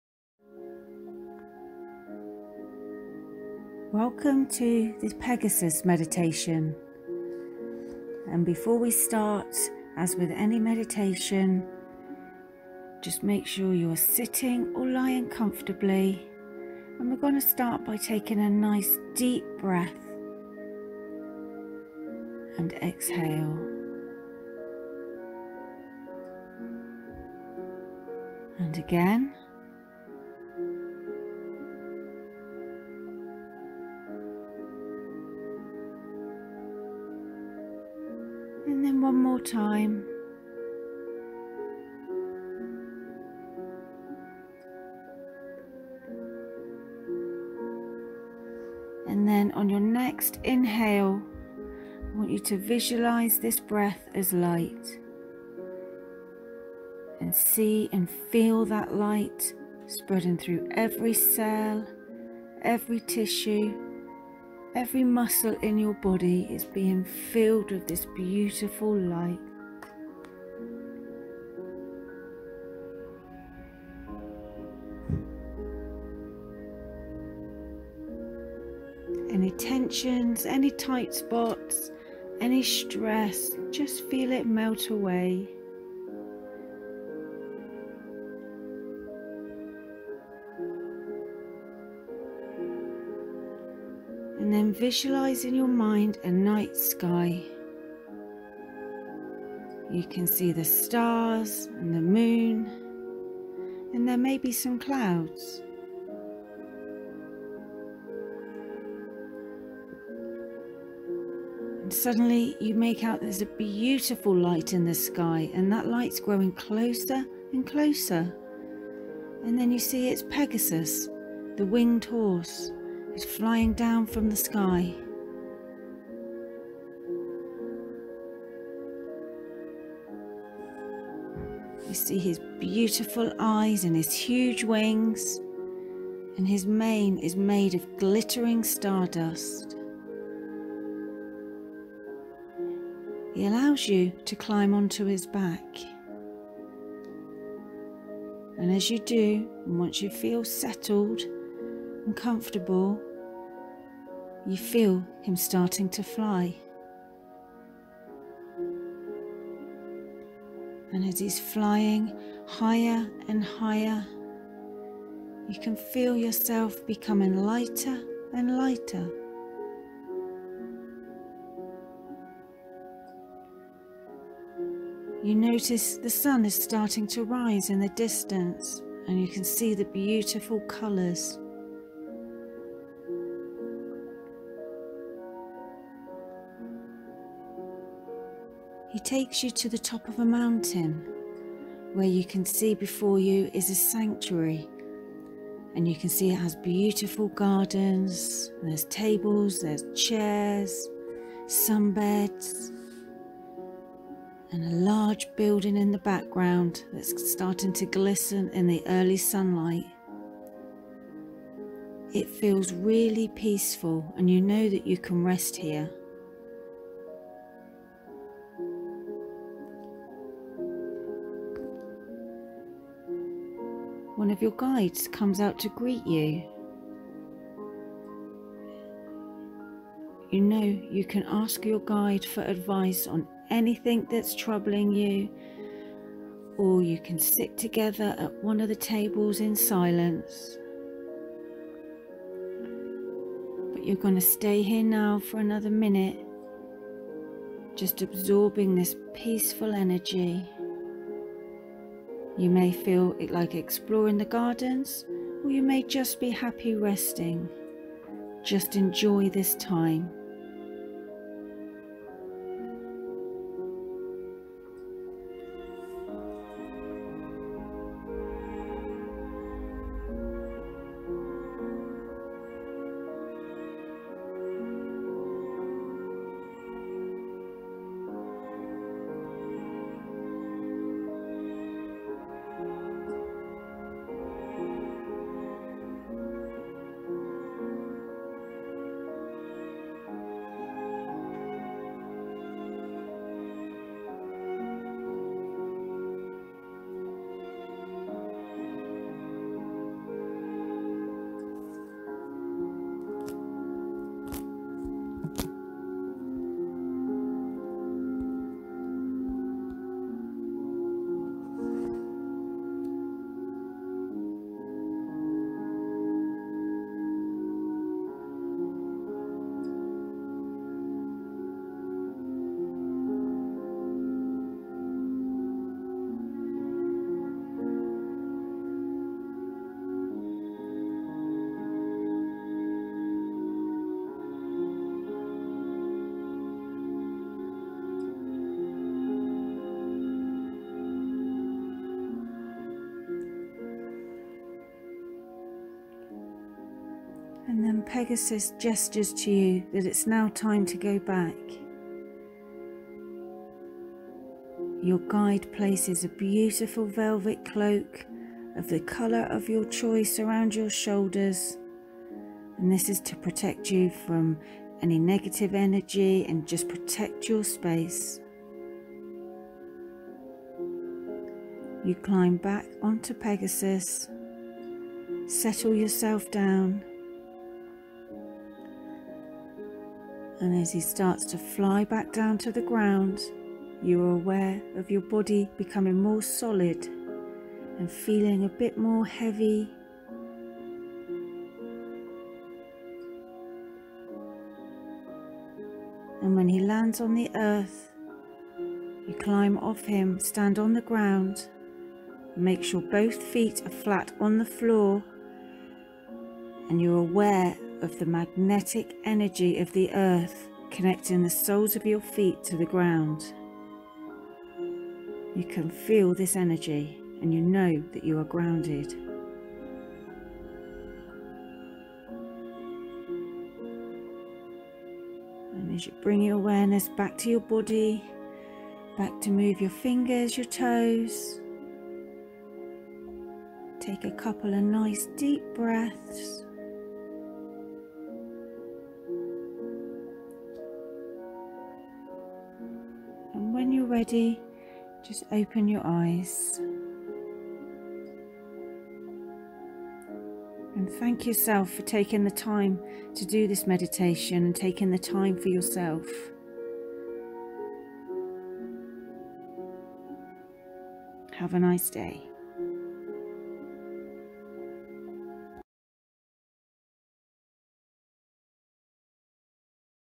Pegasus meditation
This meditation is written by Donna Maxine, spoken by Carla Samaden.

We have a huge boost of healing energy from the Great Square of Pegasus, this will help you "rise above " old patterns or limitations and brings about a sense of inner knowing. It can be very beneficial to do this short meditation.